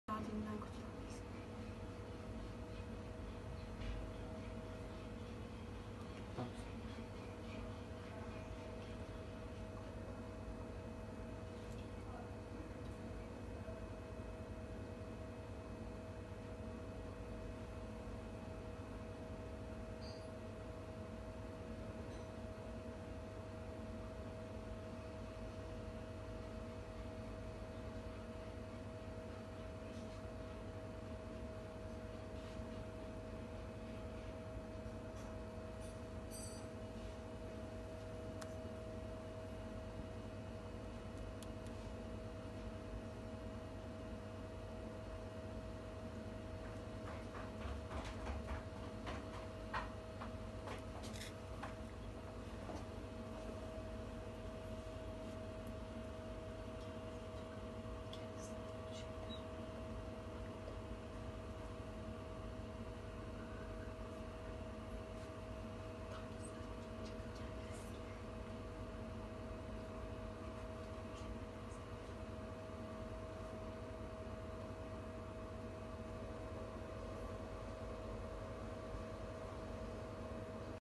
Kuyumcu soydu, kendini yakalayan polisi tebrik etti
İzmit’te bulunan bir kuyumcudan 32 bin TL değerinde ziynet eşyası çalan ve polise yakalanmamak için 5 kez kıyafet ve 4 kez de otobüs değiştiren hırsız, tüm çabalarına rağmen yakayı ele verince kendisini yakalayan polisleri tebrik etti.